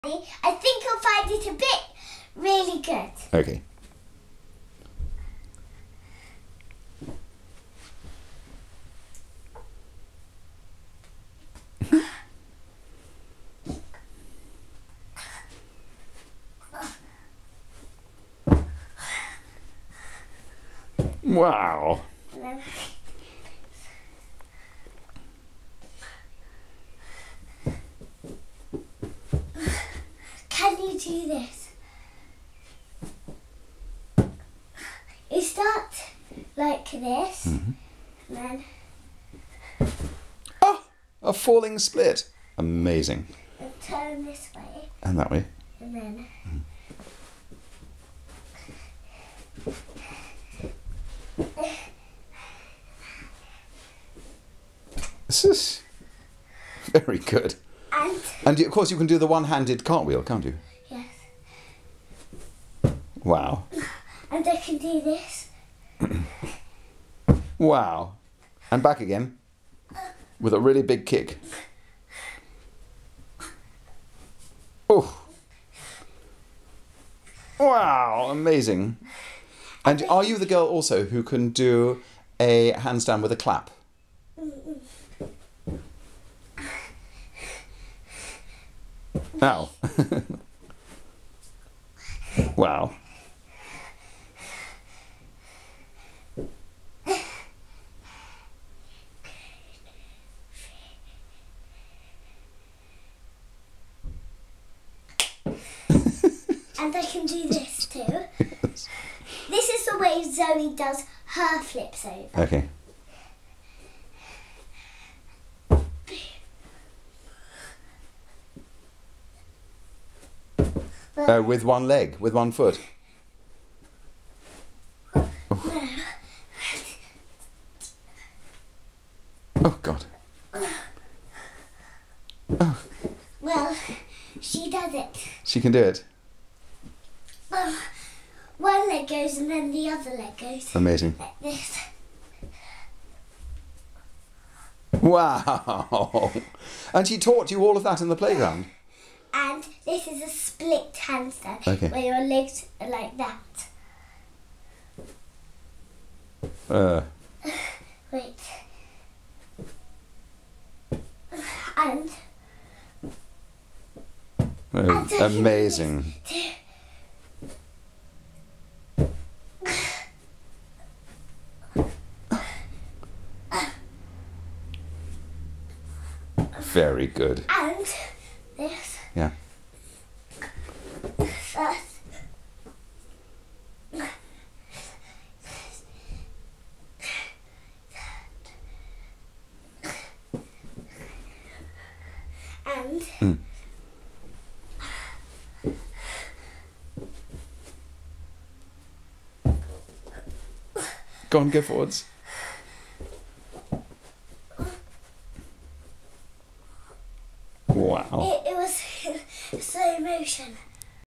Gamestheory: Sophie (just 6 years) doing flips in the front room
Sophie's friend Zoe, who goes to high-intensity gym classes during the week, has taught Sophie to do back flips, crab flips and handstands in the playground.